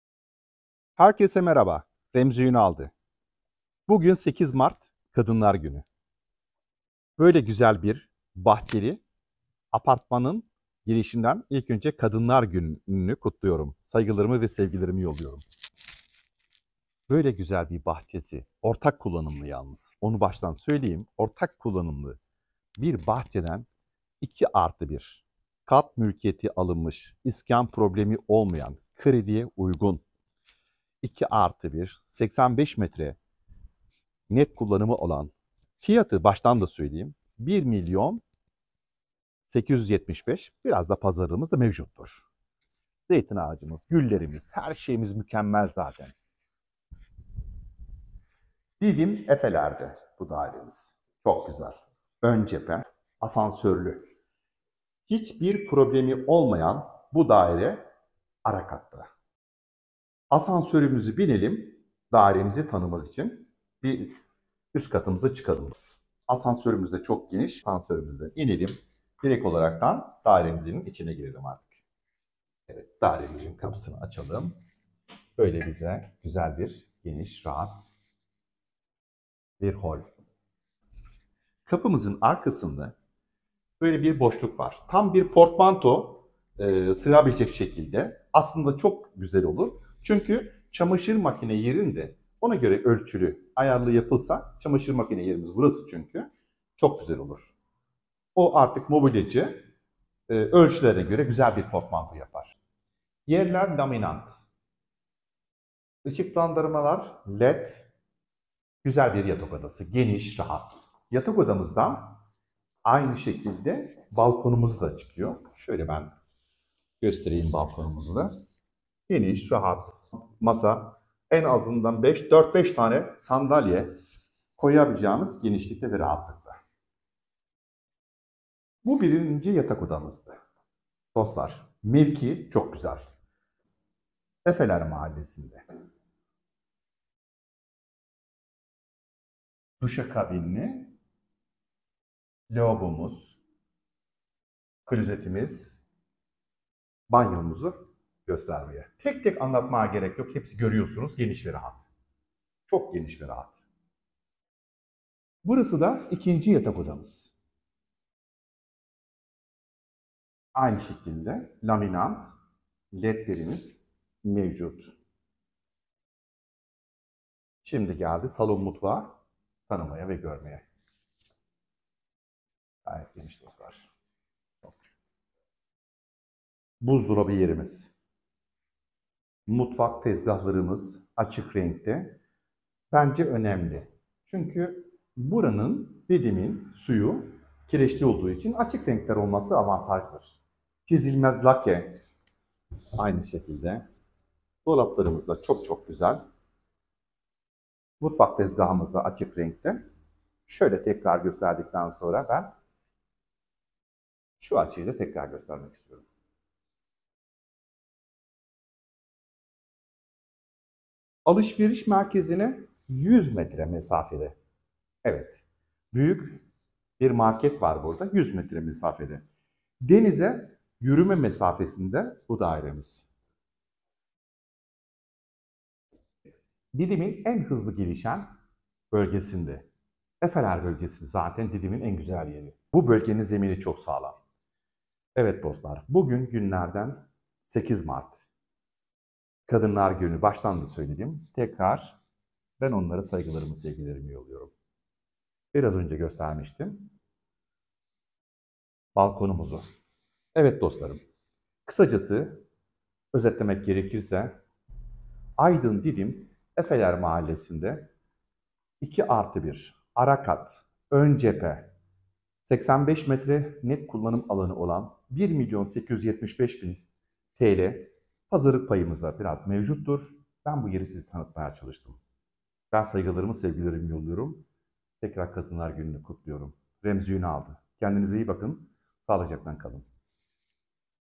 Şok şok Didim Efeler'de Asansörlü Ara Kat 2+1 Satılık Daire
Didim Efeler'de Satılık Asansörlü
Ara Kat 2+1 Daire
Detaylar için Arayın

ünaldı gayrimenkul
Remzi Ünaldı